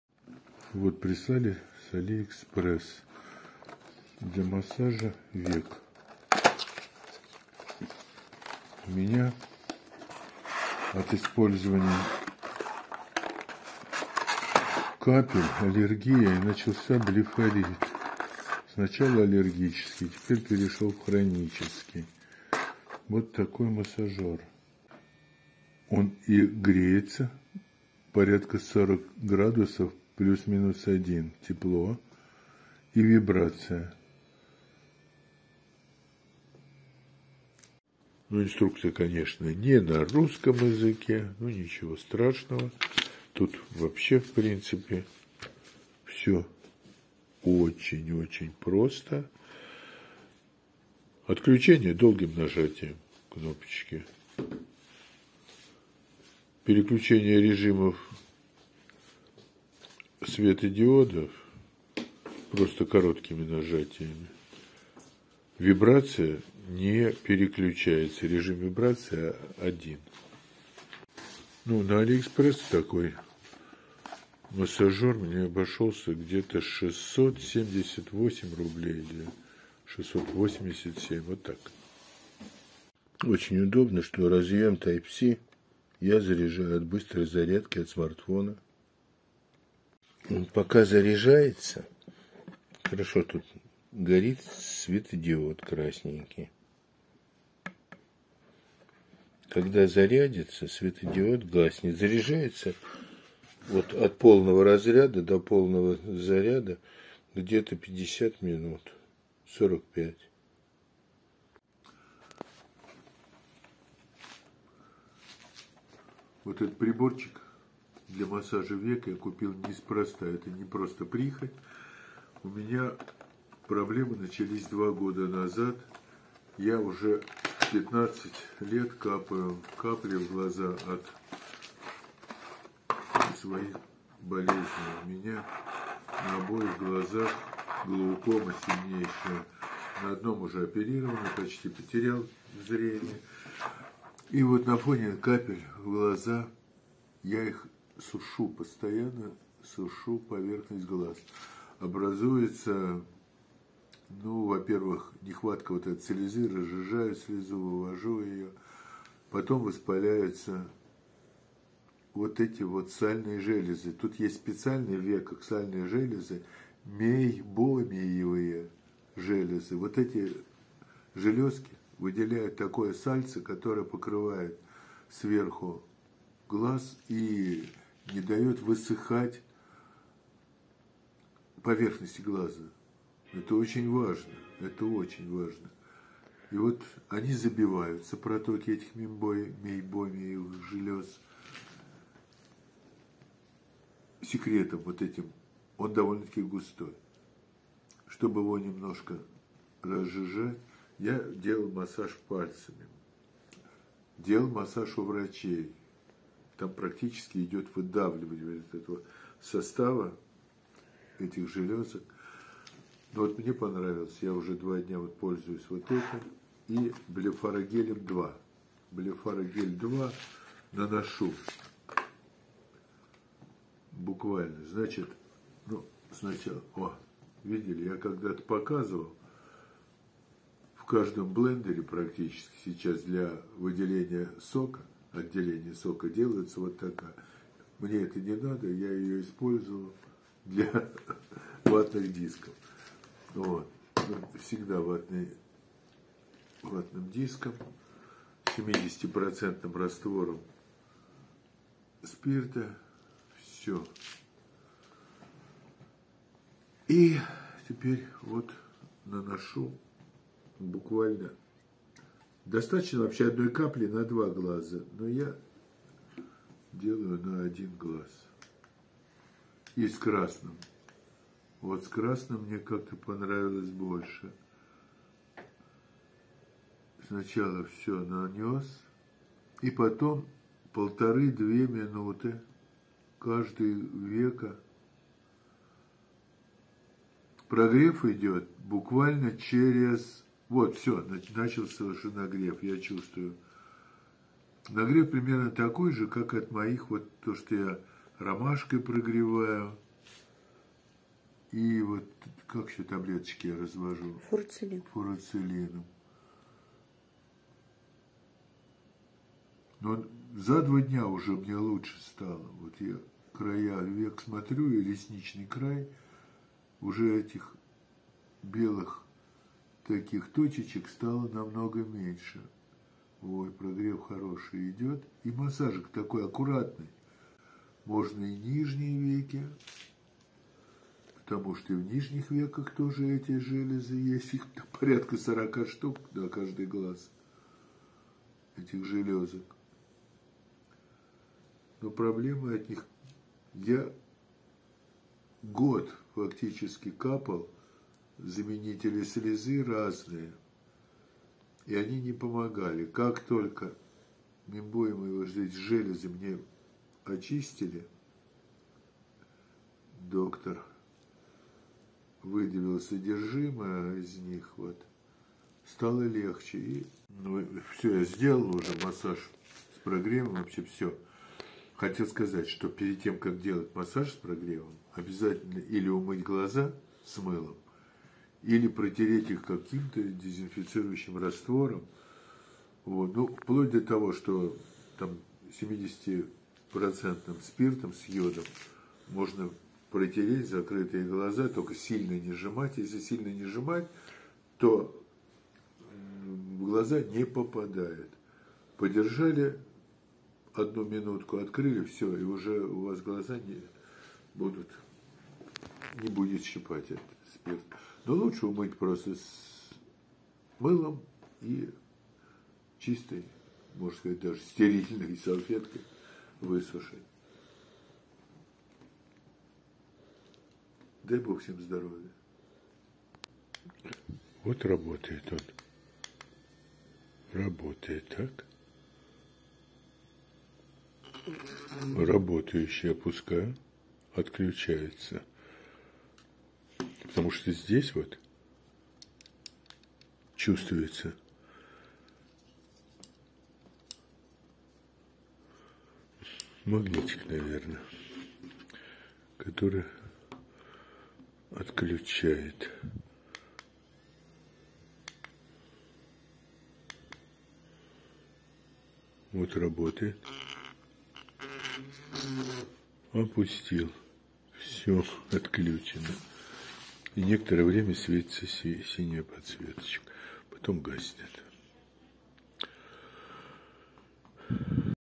Массажер для век помогает мне при блефарите и синдроме сухого глаза.
Специально подобрал массажер с функцией подогрева для облегчения эвакуации продукции сальных-мейбомиевых  желёз.уже на третий день почувствовал  положительный эффект.Массажеры подобные моему продаются на всех маркетплейсах.